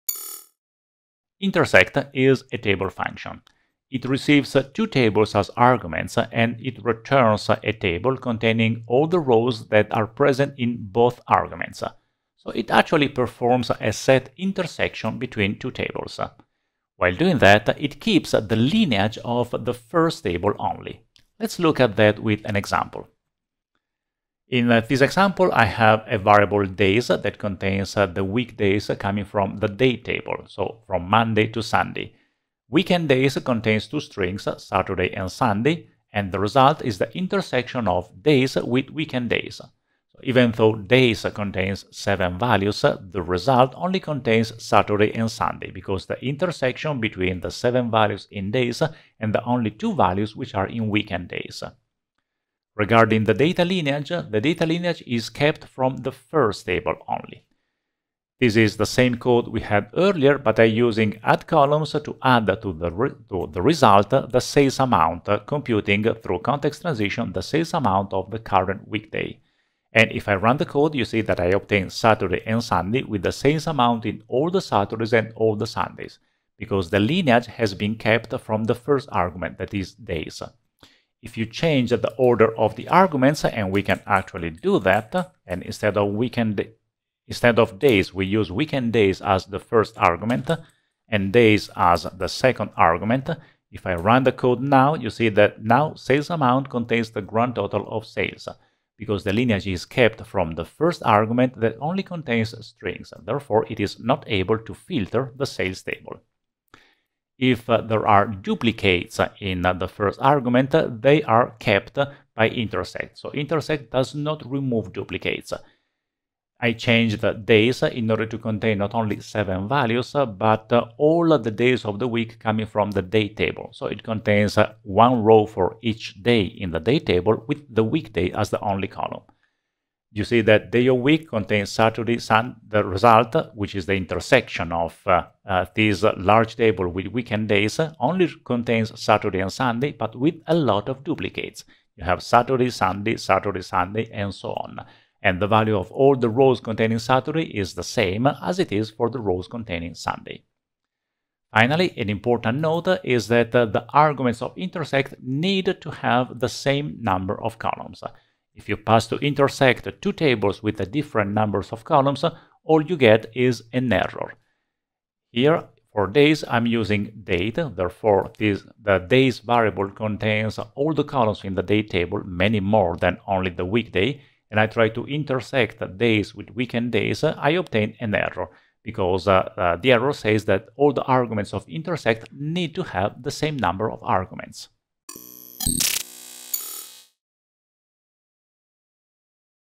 INTERSECT - DAX Guide
INTERSECT: Returns the rows of left-side table which appear in right-side table.